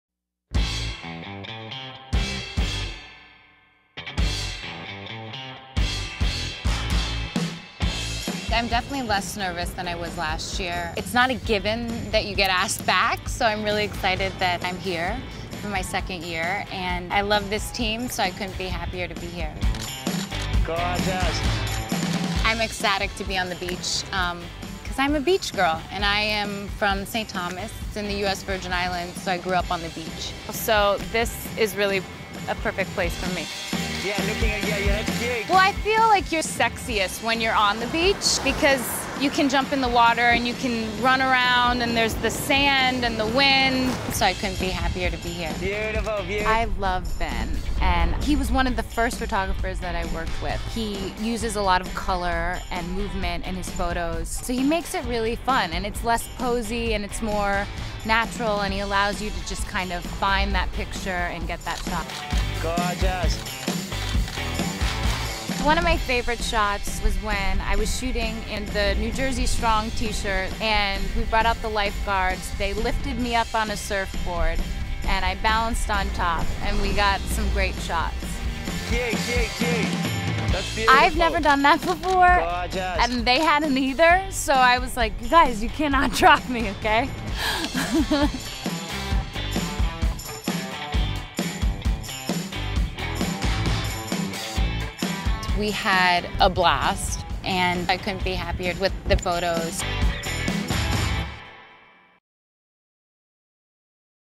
Hannah Davis Stunning Model Profile | Sports Illustrated Swimsuit
See stunning photos of Hannah Davis in Sports Illustrated Swimsuit 2014 issue. Hannah was photographed in Jersey Shore by photographer Ben Watts.